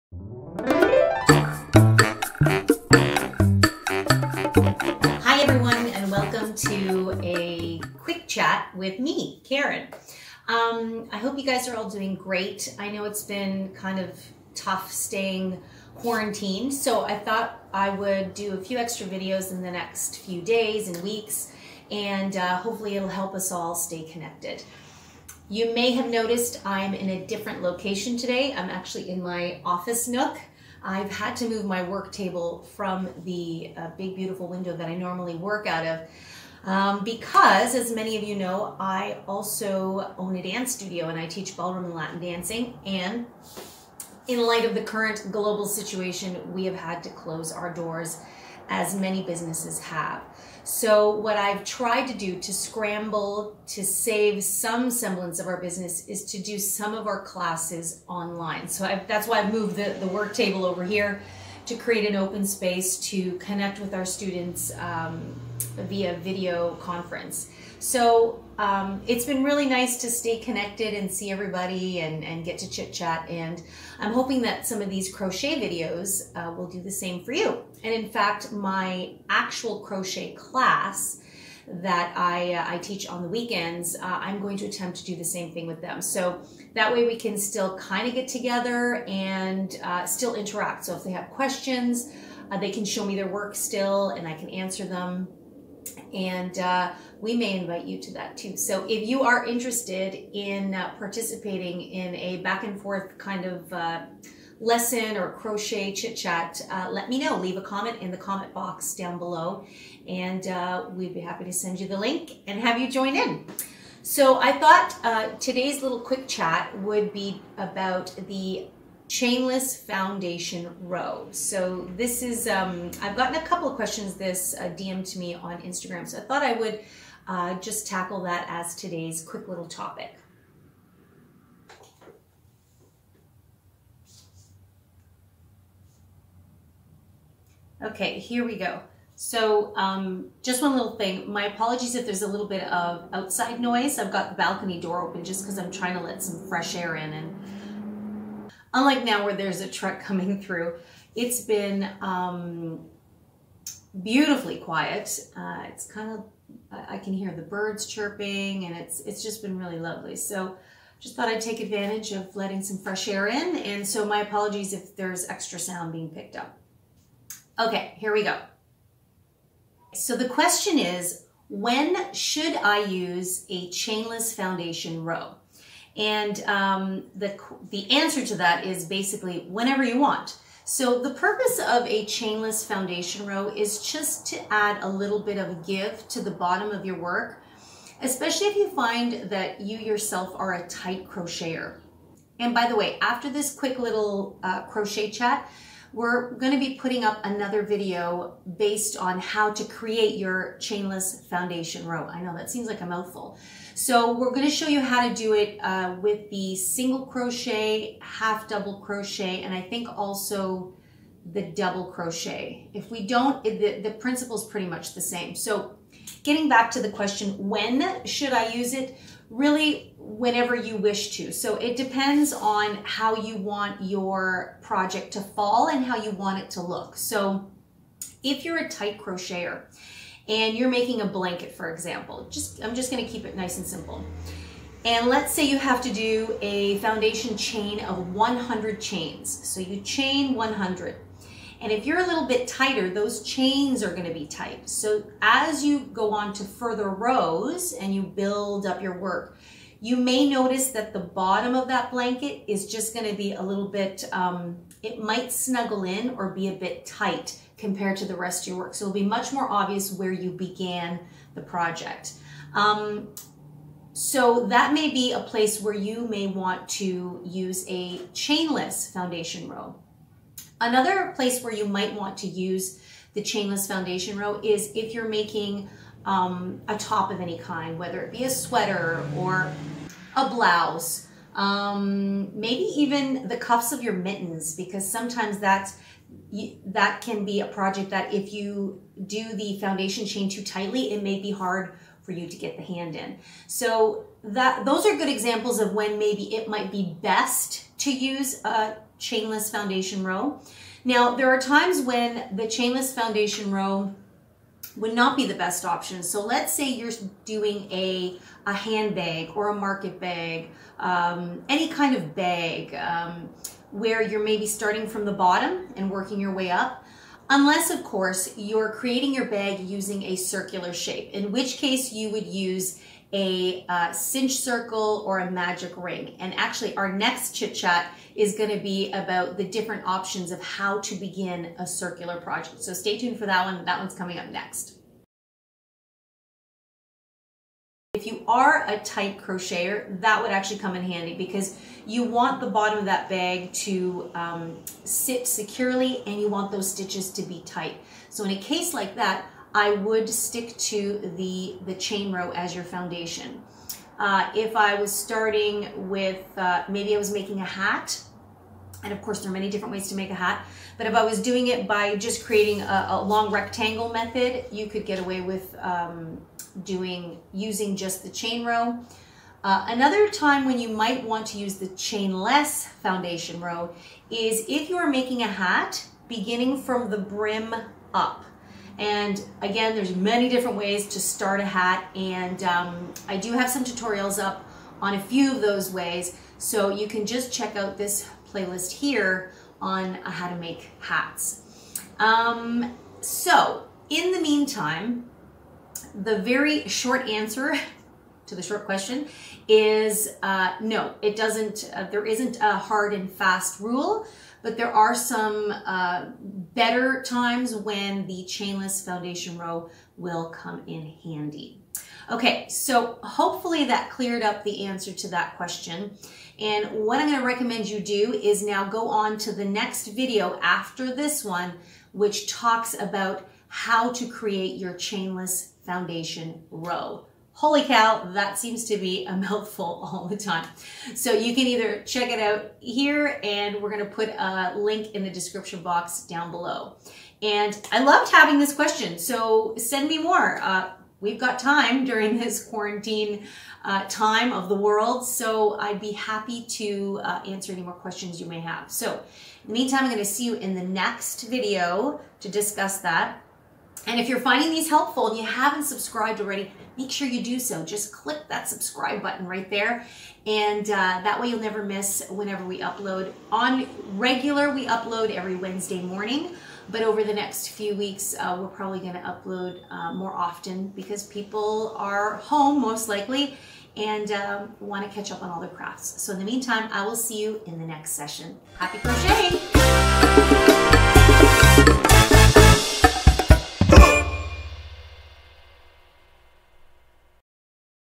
Crochet Quick Chat #1 - When should I use a Chainless Foundation Row?
Hey, hey Crochet Friends!

During this time of quarantine and uncertainty, I know many of us are staying closer to home and might be looking for some extra things crafty to take our minds off of everything. Soooo, I thought I would create an additional series called "Crochet Quick Chat" where I sit and have a short chat about some of your most common crochet questions. I hope you find this extra useful for those of you that are super beginners but I also hope it entertains you a little and helps you to feel like you are still connected at this time.

Here are a few additional links I mention in the video for reference:

Stop in and say hello!

ETSY STORE - Crochet Crafty Canada

Happy Crocheting!
-K